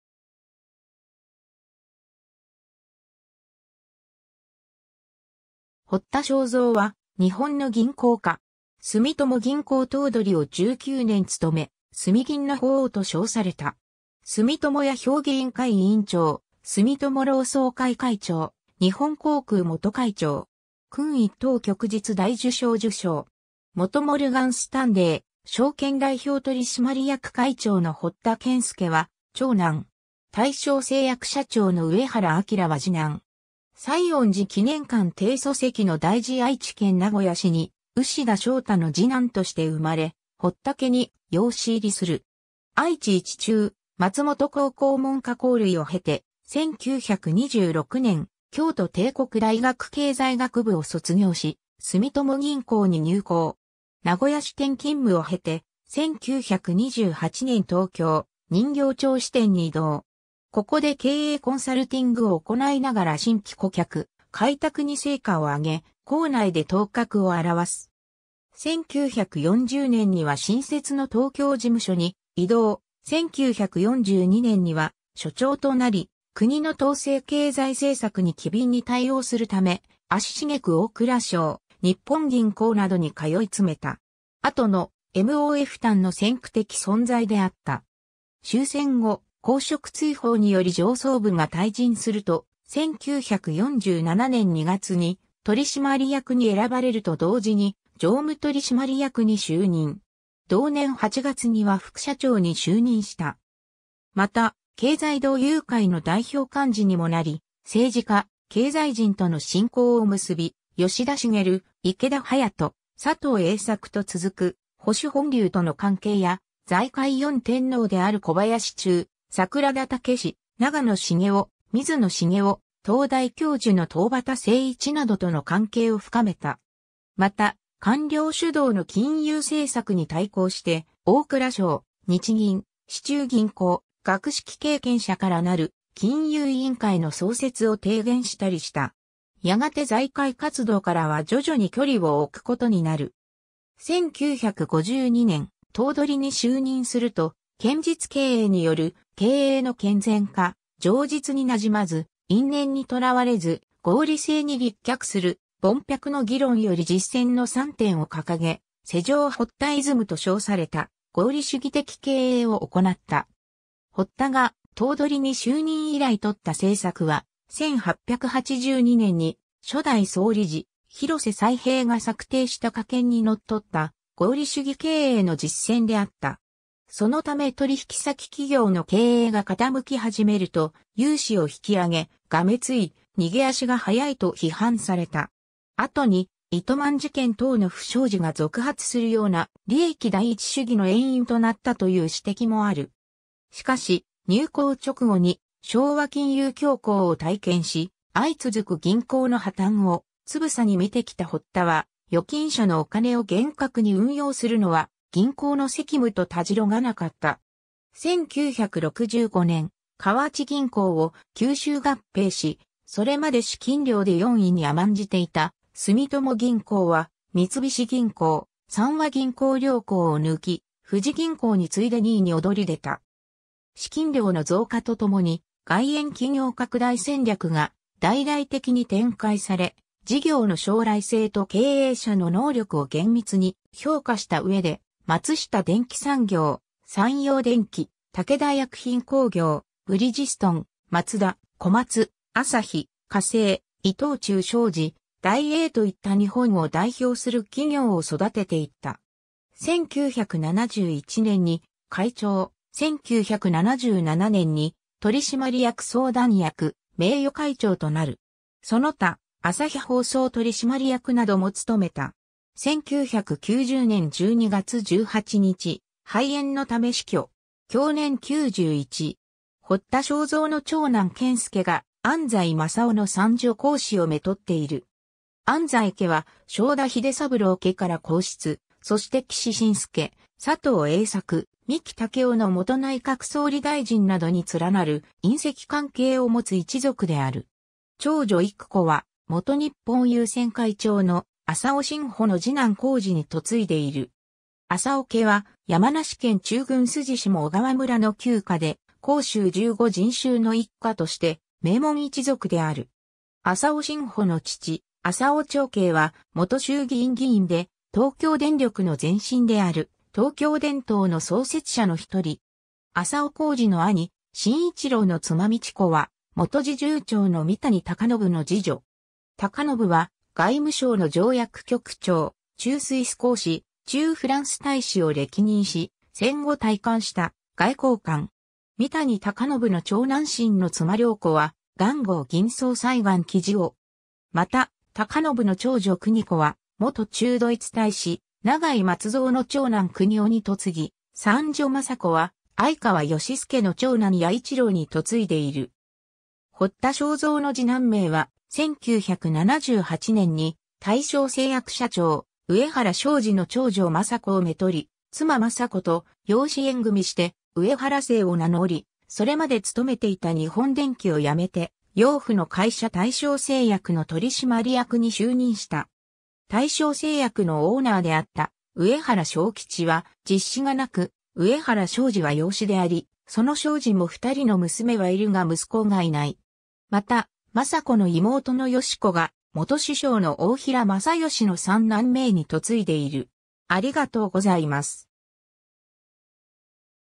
堀田庄三
堀田 庄三（ほった しょうぞう、1899年1月23日 - 1990年12月18日）は日本の銀行家。
住友銀行頭取を19年務め（1952年～1971年）、「住銀の法皇」と称された。
住友家評議員会委員長、住友老壮会会長、日本航空元会長。
勲一等旭日大綬章受章。
元モルガン・スタンレー証券代表取締役会長の堀田健介は長男。
大正製薬社長の上原明は二男。
 「 西園寺記念館」定礎石の題字（堀田庄三 揮毫） 愛知県名古屋市に牛田正太の二男として生まれ、堀田家に養子入りする。
愛知一中、松本高校文科甲類を経て、1926年京都帝国大学経済学部を卒業し、住友銀行（現・三井住友銀行）に入行。
名古屋支店勤務を経て、1928年東京・人形町支店に異動。
ここで経営コンサルティングを行いながら新規顧客開拓に成果を上げ、行内で頭角を顕す。
1940年には新設の東京事務所に異動、1942年には所長となり、国の統制経済政策に機敏に対応するため、足しげく大蔵省、日本銀行などに通いつめた。
後の“MOF担”の先駆的存在であった。
 終戦後、公職追放により上層部が退陣すると、1947年2月に取締役に選ばれると同時に常務取締役に就任。
同年8月には副社長に就任した。
また経済同友会の代表幹事（当時の役職名は当番幹事）にもなり、政治家、経済人との親交を結び、吉田茂、池田勇人、佐藤栄作と続く保守本流との関係や、財界四天王である小林中、桜田武、永野重雄、水野成夫、東大教授の東畑精一などとの関係を深めた。
また、官僚主導の金融政策に対抗して、大蔵省・日銀・市中銀行・学識経験者からなる金融委員会の創設を提言したりした。
やがて財界活動からは徐々に距離を置くことになる（住友銀行が財界活動に復帰するのは1986年に磯田一郎が経団連副会長に就任するまで待たなければならなかった）。
 1952年、頭取に就任すると、「堅実経営による経営の健全化」「情実になじまず、因縁にとらわれず、合理性に立脚する」「凡百の議論より実践」の三点を掲げ、世上堀田イズムと称された合理主義的経営を行った。
 堀田が頭取に就任以来取った政策は、1882年に初代総理事広瀬宰平が策定した家憲（「営業の信用を重んじ確実をむねとする」「浮利にはしり、軽進すべからず」などが謳われている「家法」）にのっとった合理主義経営の実践であった。
そのため取引先企業の経営がかたむき始めると融資を引き上げ、「がめつい」「逃げ足が速い」と批判された。
後にイトマン事件等の不祥事が続発するような、利益第一主義の遠因となったという指摘もある。
 しかし入行直後に昭和金融恐慌を体験し、相続く銀行の破綻をつぶさに見てきた堀田は、「預金者のお金を厳格に運用するのは銀行の責務」とたじろがなかった。
1965年、河内銀行を吸収合併し、それまで資金量で四位に甘んじていた住友銀行は、三菱銀行、三和銀行(両行とも現三菱UFJ銀行)両行を抜き、富士銀行に次いで二位に躍り出た。
資金量の増加と共に外延企業拡大戦略が大々的に展開され、事業の将来性と経営者の能力を厳密に評価した上で、松下電器産業、三洋電機、武田薬品工業、ブリヂストン、マツダ、コマツ、旭化成、伊藤忠商事、ダイエーといった日本を代表する企業を育てていった。
 1971年に会長、1977年に取締役相談役・名誉会長となる。
その他、朝日放送取締役なども務めた。
 1990年12月18日、肺炎のため死去。
享年91。
 堀田庄三の長男健介が安西正夫の三女公子を娶っている。
安西家は、正田英三郎家から皇室、そして岸信介、佐藤栄作、三木武夫の元内閣総理大臣などに連なる姻戚関係を持つ一族である。
 長女育子は、元日本郵船会長の浅尾新甫の二男浩二に嫁いでいる。
浅尾家は山梨県中郡筋下小河村の旧家で「甲州十五人衆」の一家として名門一族である。
浅尾新甫の父浅尾長慶は元衆議院議員で、東京電力の前身である東京電燈の創設者の1人。
浅尾浩二の兄新一郎の妻道子は、元侍従長の三谷隆信の二女。
隆信は外務省の条約局長、駐スイス公使、駐フランス大使を歴任し、戦後退官した外交官。
三谷隆信の長男信の妻涼子は、元興銀総裁岸喜二雄。
また隆信の長女邦子は、元駐ドイツ大使永井松三の長男邦夫に嫁ぎ、三女正子は、鮎川義介の長男弥一郎に嫁いでいる。
 堀田...